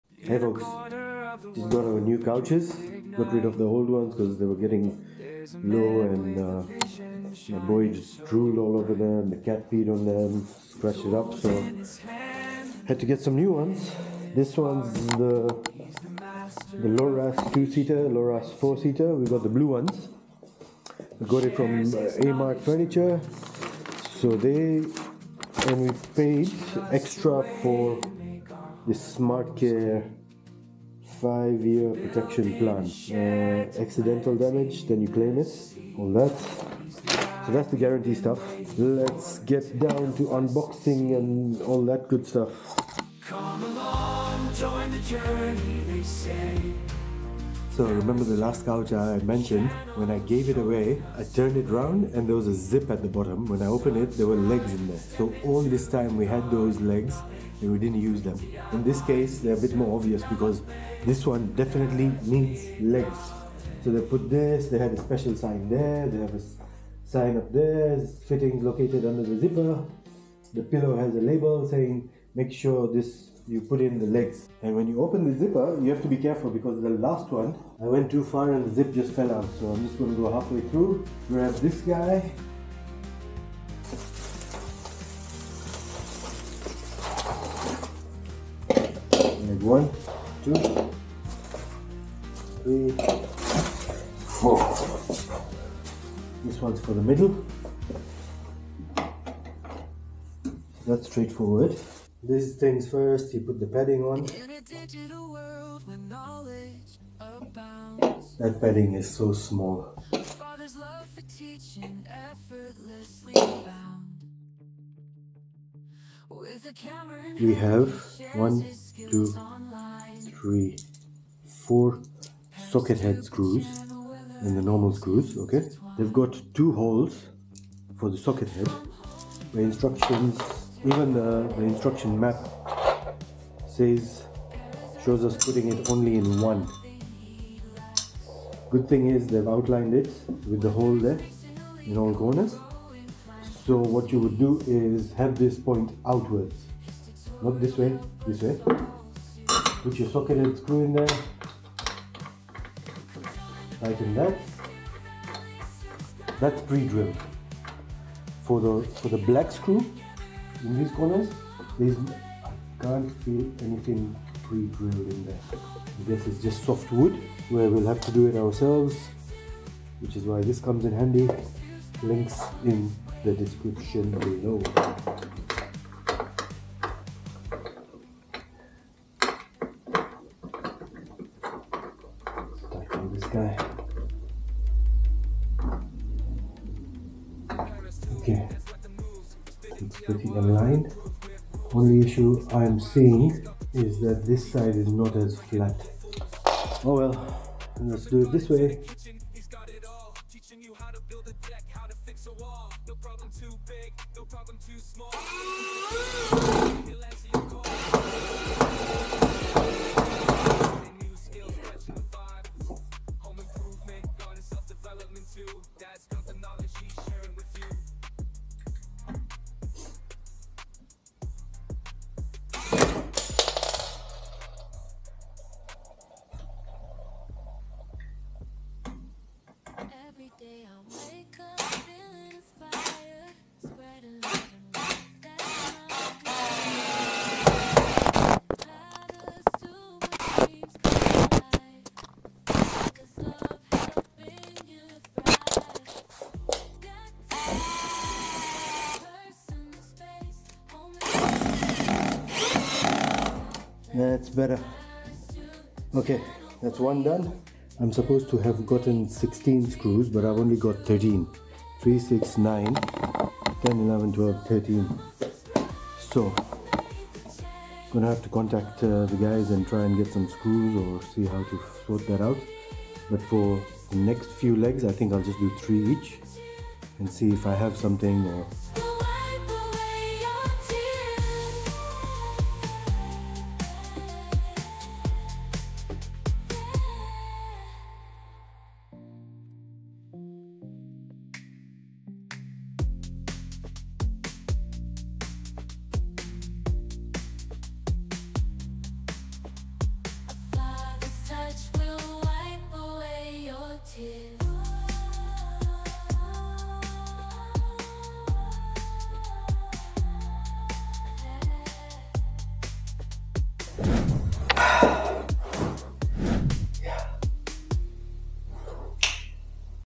Wood Furniture Legs Installation Guide (Sofa Feet for Loras from Amart Furniture)
Links to Instruction manual and more coming soon.

- Want custom Merch?